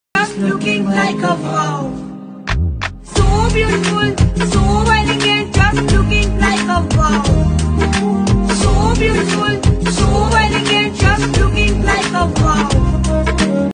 They always look like a WOW!
Dive into the tranquil world of our aquarium with this snippet! Meet our adorable turtle, basking under the warmth of his lamp and gliding through crystal clear waters. Perfect for a quick moment of peace in your day.